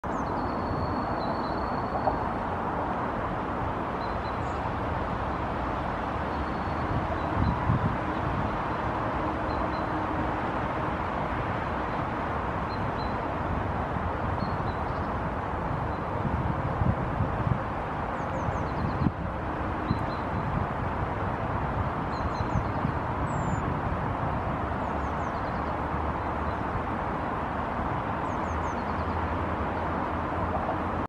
Great Tit foraging on the lawn
Great Tit (Parus major) foraging on the lawn.

Talitiainen (Parus major) etsii syötävää nurmikolta.